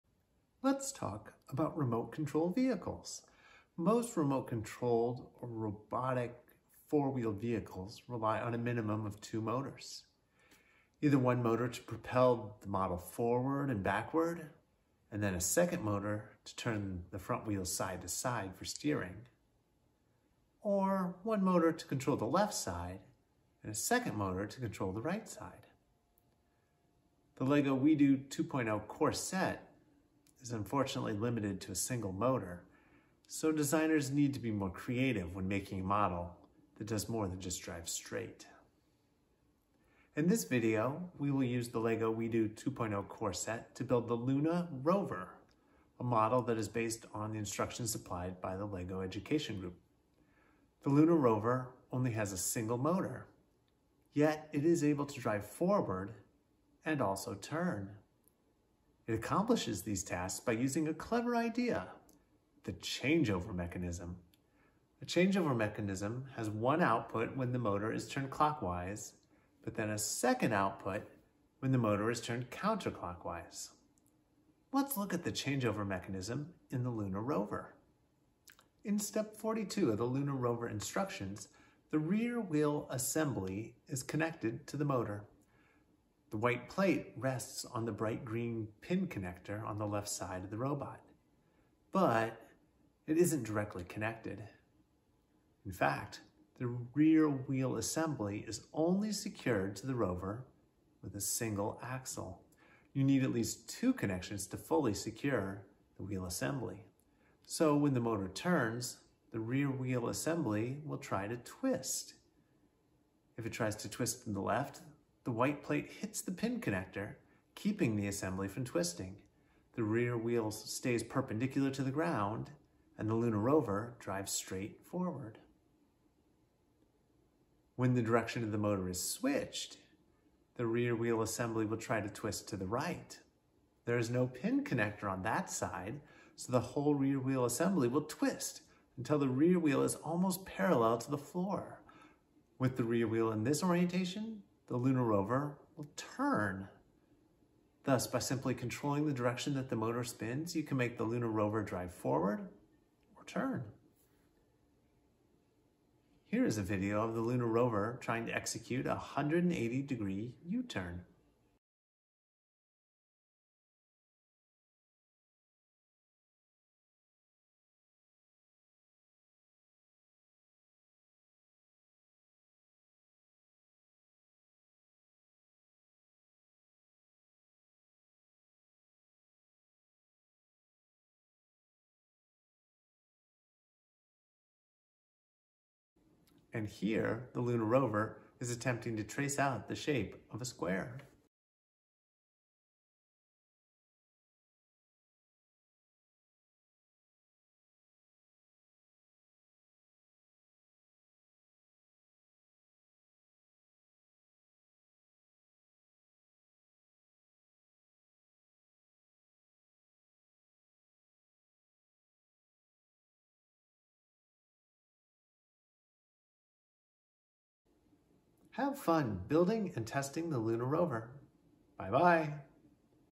LEGO WeDo 2.0 Luna Rover
This video describes the Luna Rover, based on the instructions from the LEGO Education group.  It uses the parts from the LEGO WeDo 2.0 core set (#45300).  The Luna Rover uses a changeover mechanism to allow a single motor to either drive the model forward or turn it in a circle.